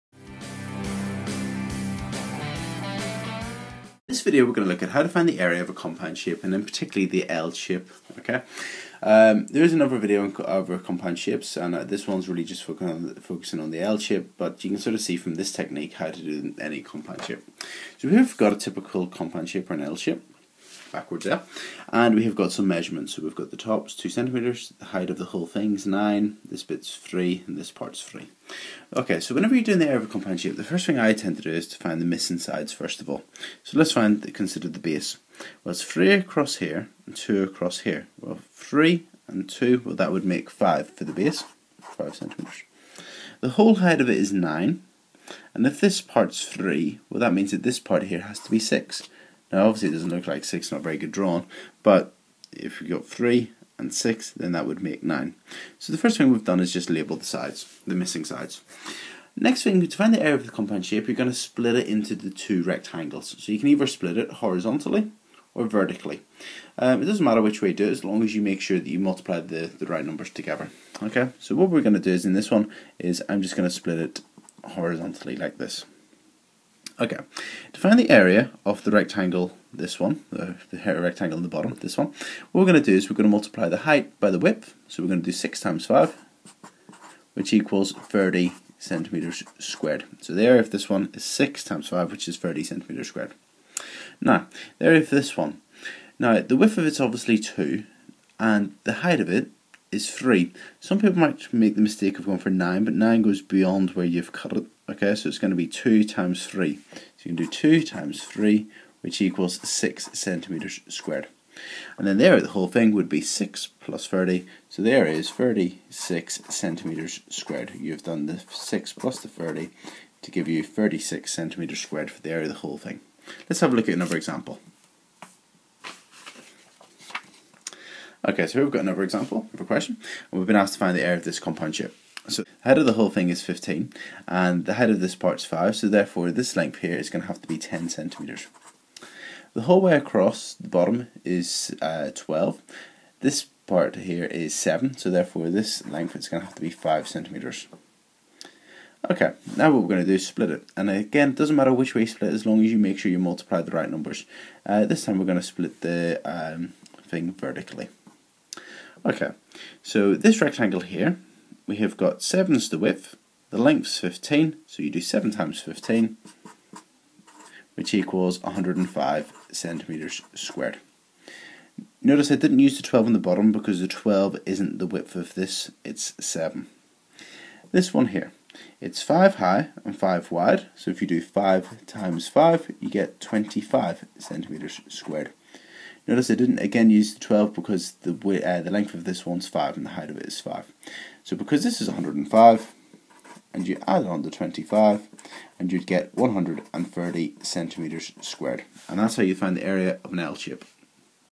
Area of an L-shape - Corbettmaths
Corbettmaths - This video shows you how to find the area of a common type of compound shape, the L-shape.